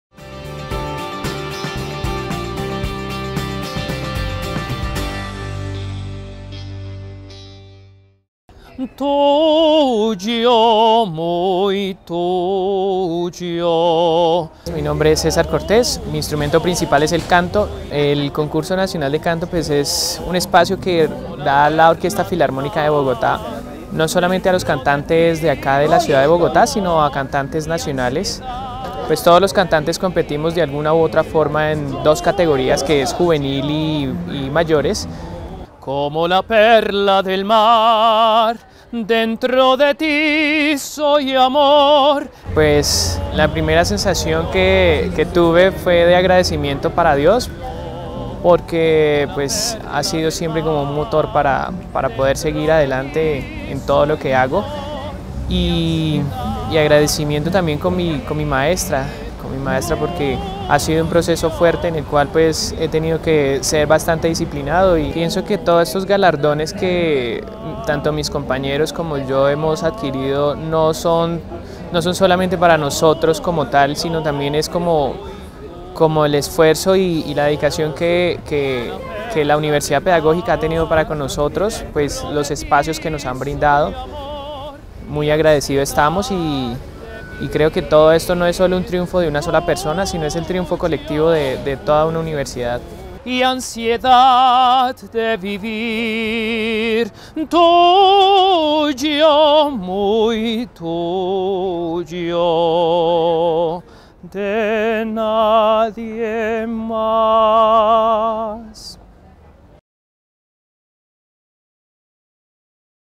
César Cortés, galardonado en el Concurso Nacional de Canto
El Departamento de Educación Musical se enorgullece en compartir con la comunidad universitaria los reconocimientos y premios obtenidos por los miembros de la Licenciatura en Música de la UPN.